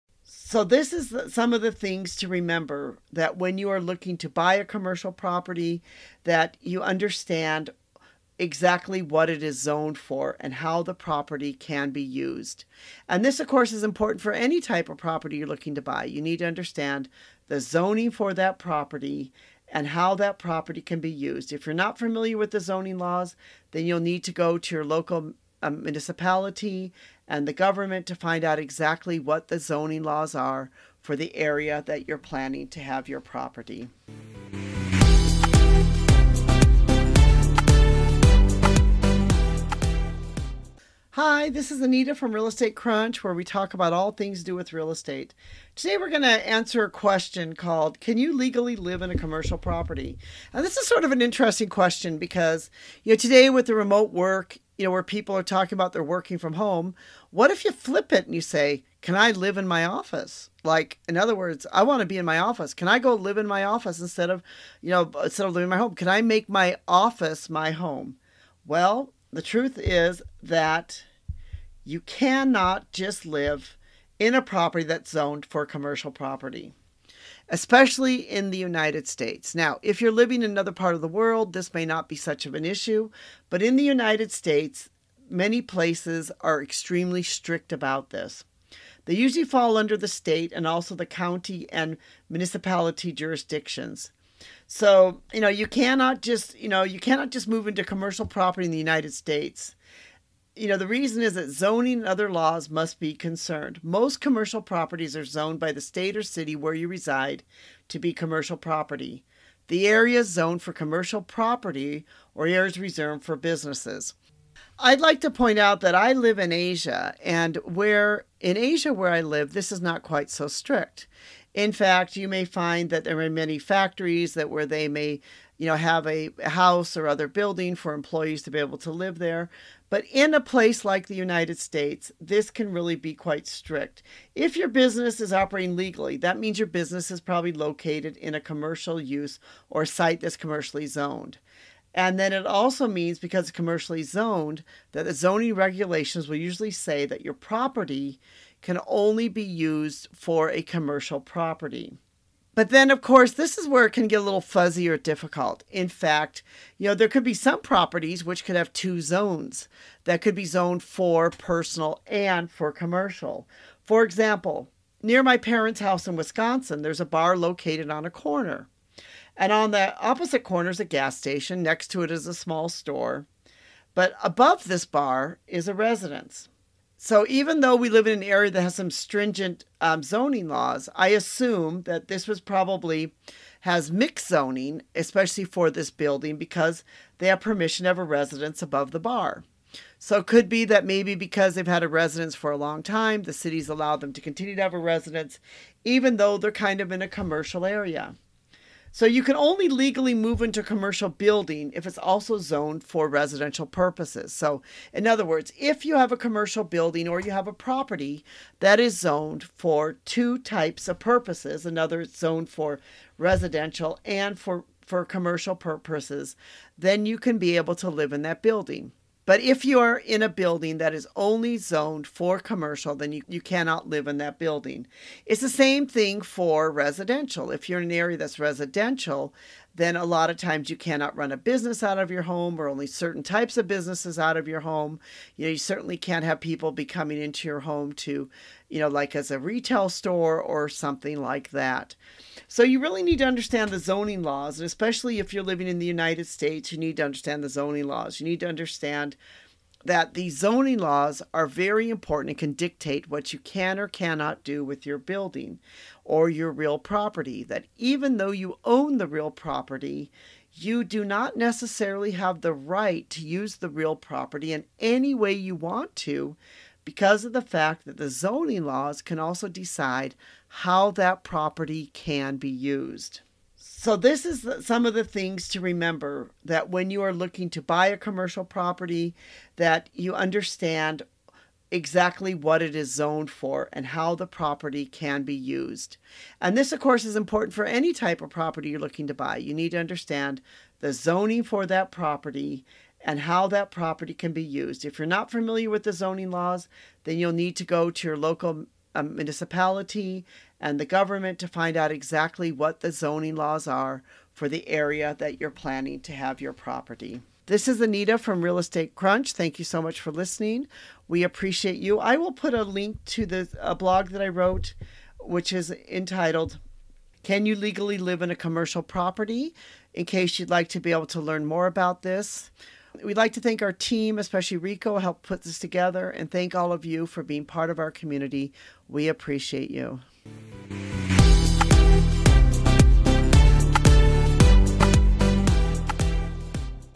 Can You Legally Live In A Commercial Property?
Have you ever wondered if it's legal to live on a commercial property? Tune in as our guest Anita shares her insights on zoning laws and why it matters for your living situation!

You can discover more about Can You Legally Live In A Commercial Property? by clicking